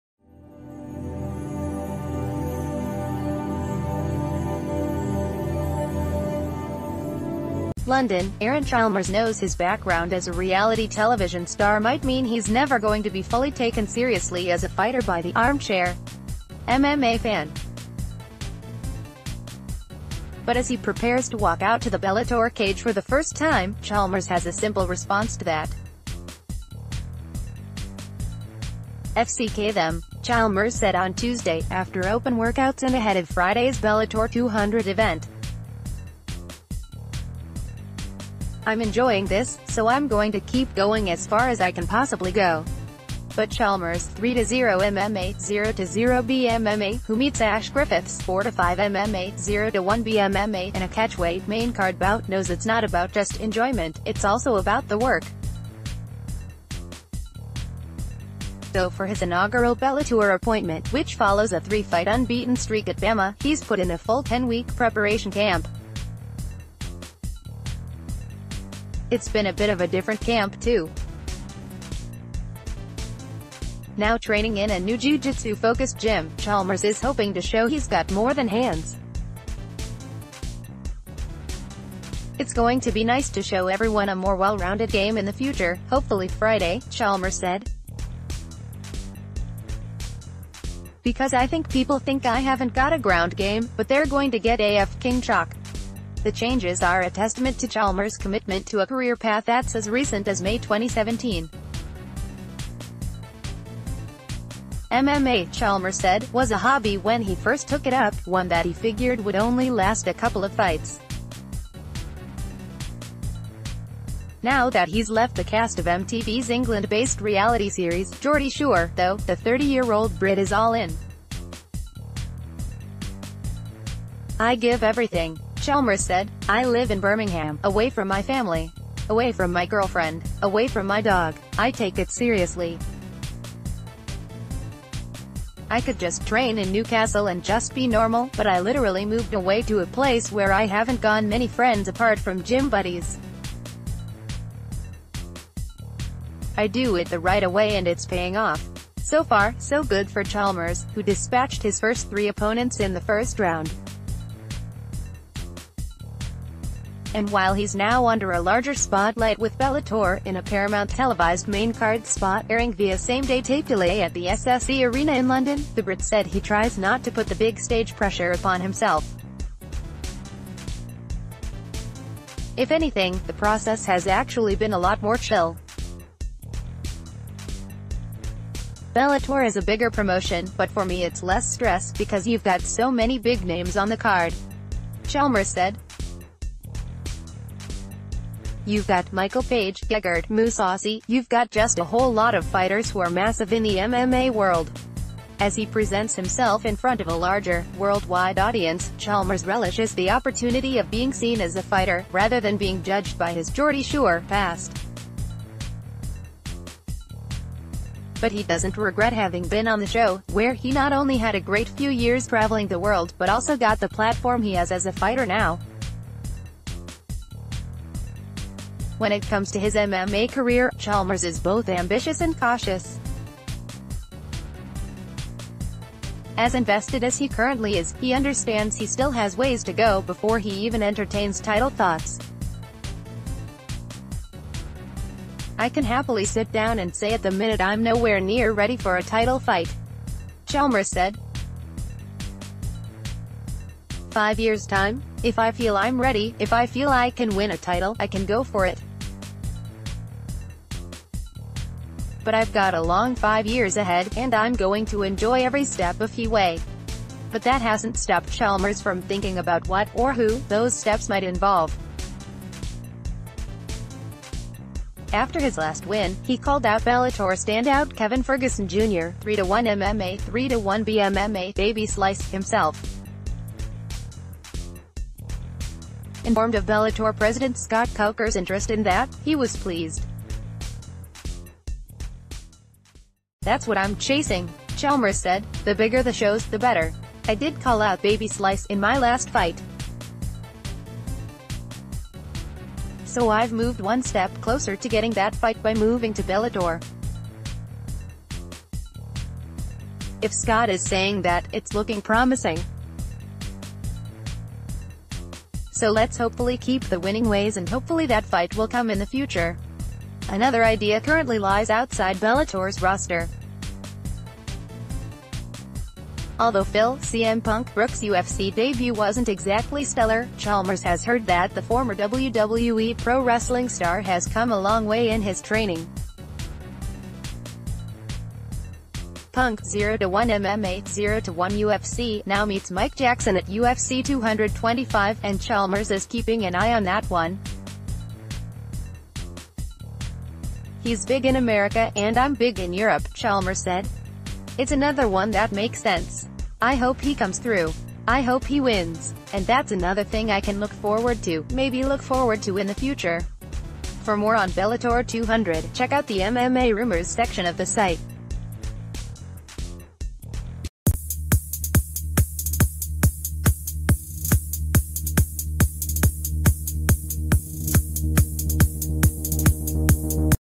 Breaking News | CM Punk, 'Baby Slice,' titles: Aaron Chalmers on goals after causing a 'f-cking sho
Breaking News | CM Punk, 'Baby Slice,' titles: Aaron Chalmers on goals after causing a 'f-cking shock' at Bellator 200

LONDON – Aaron Chalmers knows his background as a reality-television star might mean he’s never going to be fully taken seriously as a fighter by the “armchair” MMA fan. But as he prepares to walk out to the Bellator cage for the first time, Chalmers has a simple response to that. “F-ck them,” Chalmers said on Tuesday, after open workouts and ahead of Friday’s Bellator 200 event. “I’m enjoying this, so I’m going to keep going as far as I can possibly go.” But Chalmers (3-0 MMA, 0-0 BMMA), who me...